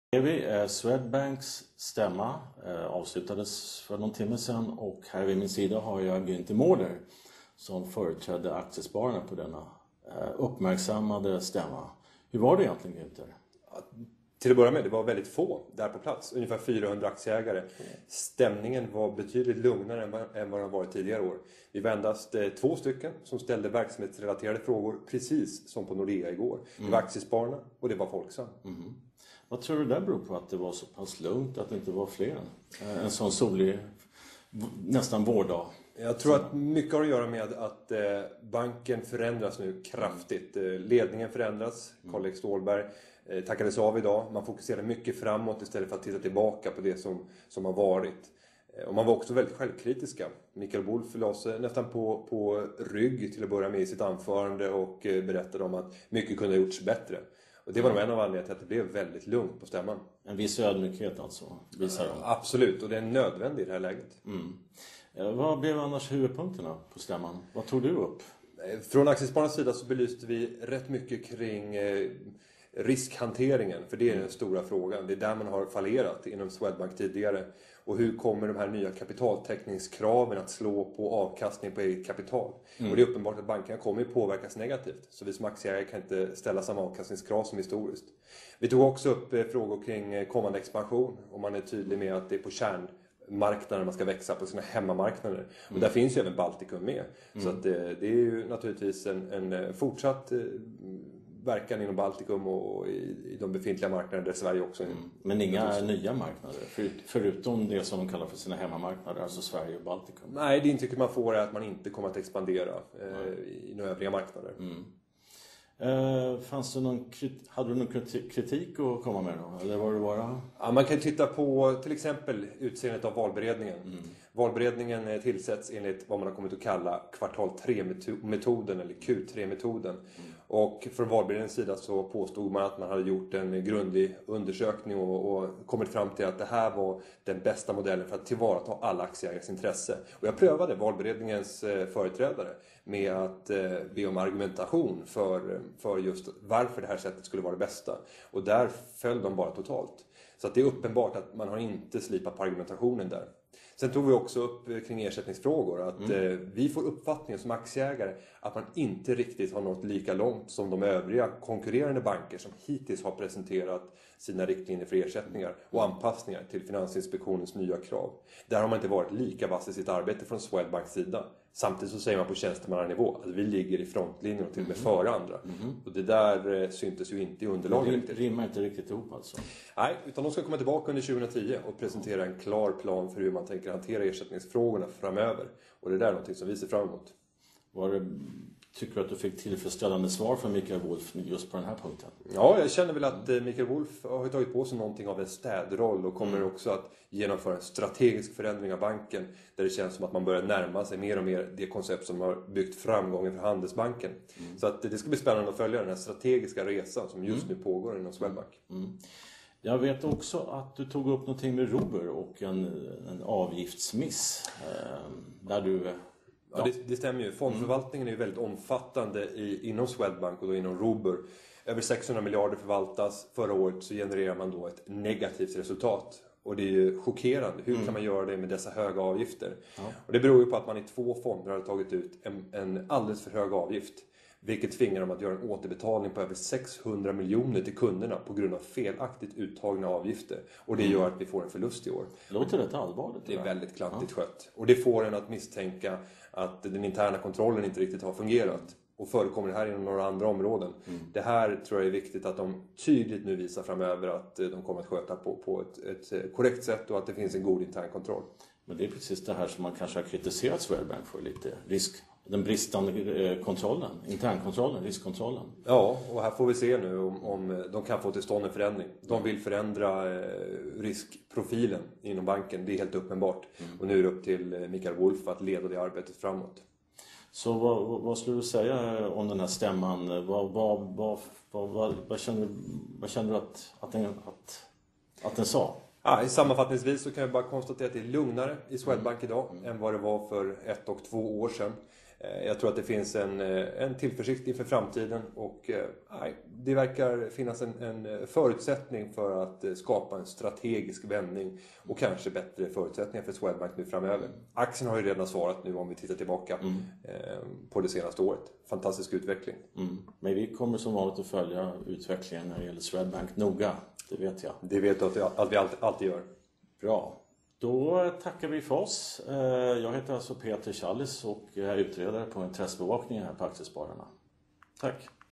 Aktiespararna om Swedbanks årsstämma 2010
Günther Mårder, Aktiespararnas VD, kommenterar Swedbanks stämma, den 26 mars 2010. Peter Challis, utredare vid Aktiespararnas intressebevakning och bisittare på stämman, är frågeställare.